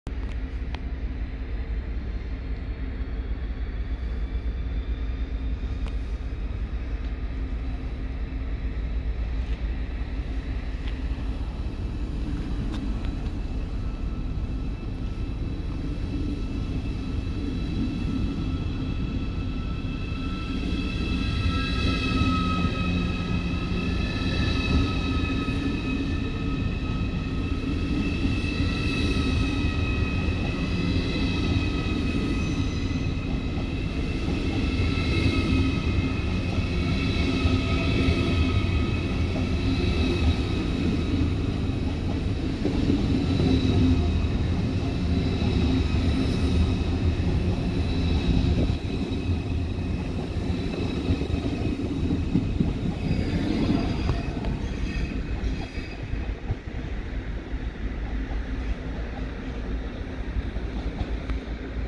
Ausfahrt ICE 502 nach Berlin Gesundbrunnen
Ausfahrt ICE 4 Baureihe 412 aus dem Münchner Hauptbahnhof als ICE 502 nach Berlin Gesundbrunnen vielen Dank fürs zuschauen ich würde mich sehr freuen wenn ihr den Kanal abonniert Glocke aktiviert bis zum nächsten Mal